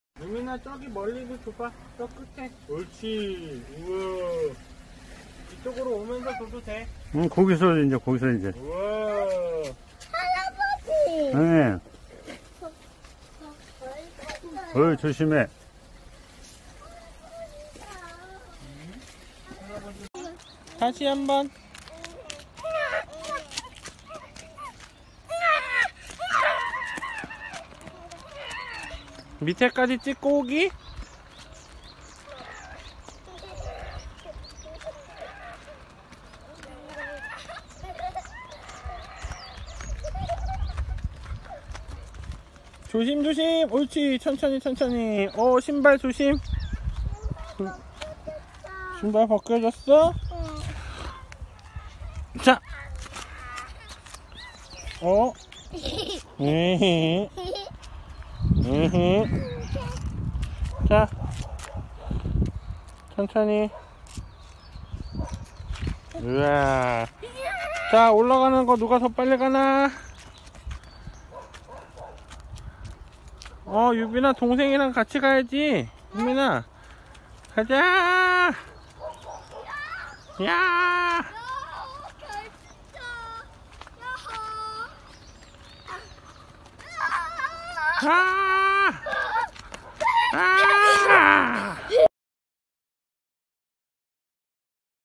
바우새미농원 텃밭농사 나이뽀 주말 물주기 정원가꾸기 손자들놀이터 시골집
2024년6월1일
(토요일)
주말시간
큰손자:박유민
작은손자:박유빈
바우새미농원
시골집에
들어와서
정원에 있는 꽃화분에
물주고
바우새미농원서
달리기
연습한다